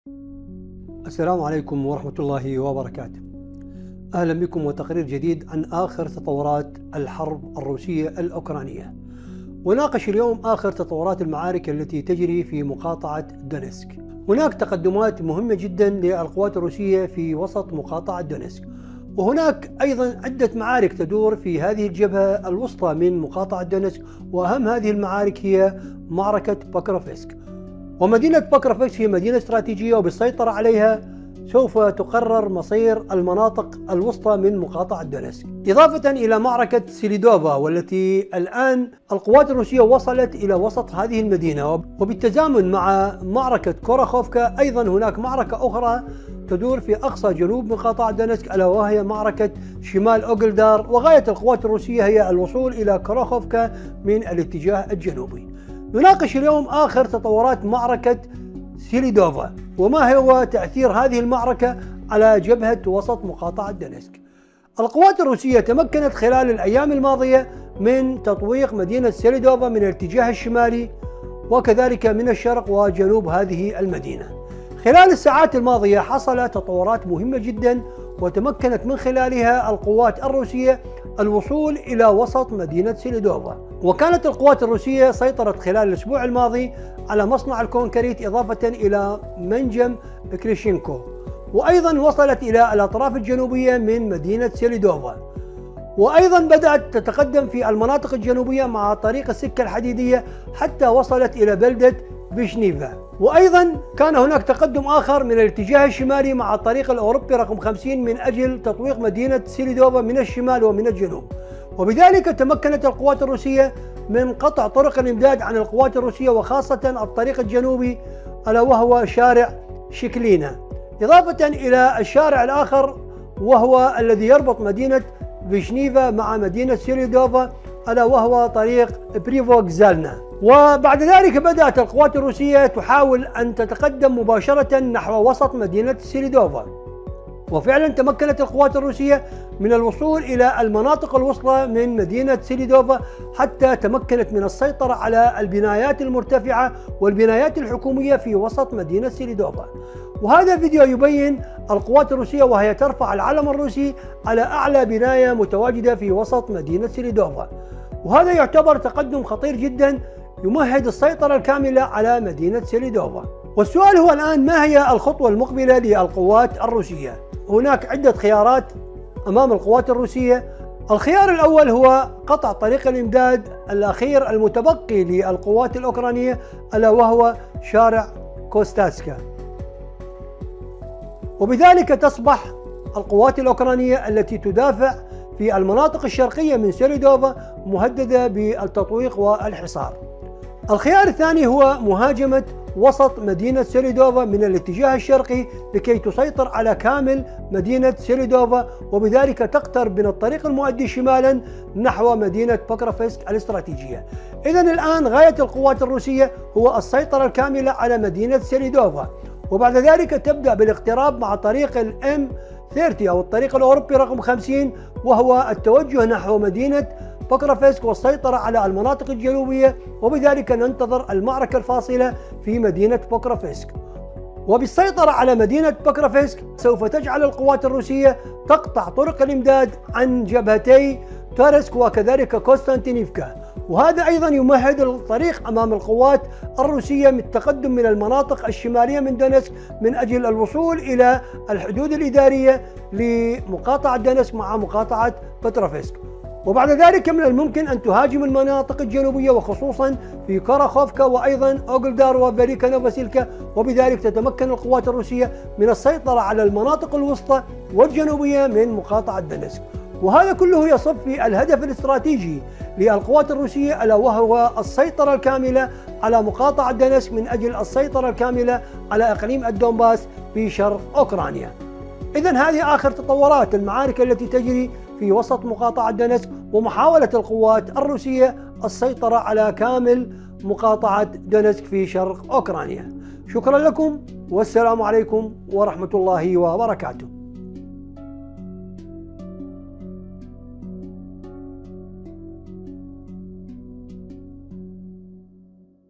معركة سليدوفا.. الجيش الروسي يقتحم "بوابة العبور" نحو بكروفسك
واصلت القوات الروسية هجماتها المكثفة في مقاطعة دونيتسك على عدة جبهات، من الشمال إلى الجنوب، محققةً تقدمًا ملحوظًا في الأيام الأخيرة بالوصول إلى وسط مدينة سليدوفا، وهو موقع استراتيجي يشكل محورًا رئيسيًا للتوسع نحو مدينة بكروفسك.